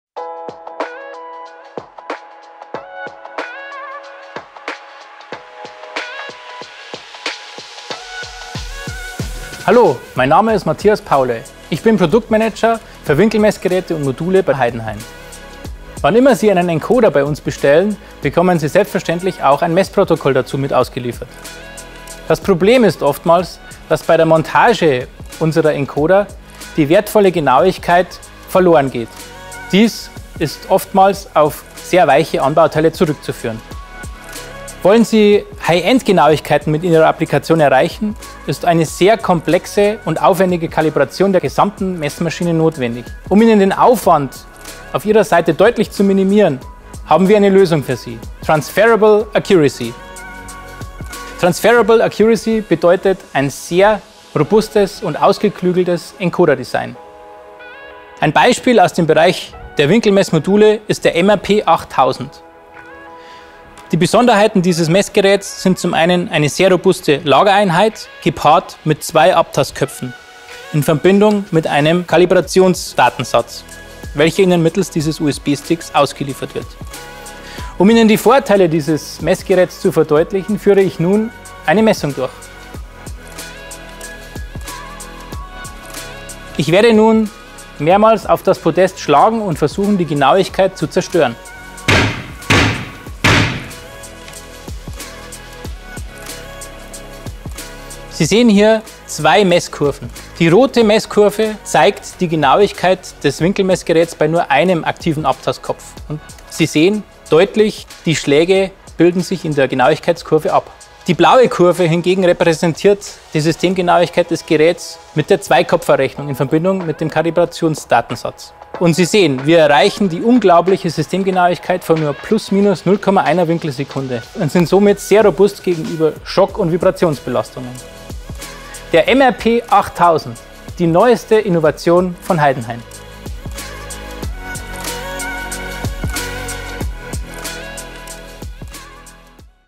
TRANSFERABLE ACCURACY – Die Messgeräte-Genauigkeit für Ihre Applikation erhalten
Dank TRANSFERABLE ACCURACY bringt das Winkelmessmodul MRP 8000 von HEIDENHAIN seine spezifizierte Genauigkeit einfach und sicher in Ihre Applikation. Ohne große Aufwände für Montage sowie Justage und ohne Abgleich von Einzelkomponenten ist das MRP 8000 die perfekte Lösung für hochgenaue Rundachsen und eine interessante Alternative zu Luftlagern. Sie können sich ganz auf Ihre Kernkompetenzen konzentrieren und hochgenaue Maschinen für die Halbleiter- und Elektronikfertigung entwickeln.

Mehr Informationen zur TRANSFERABLE ACCURACY und zum MRP 8000 von HEIDENHAIN finden Sie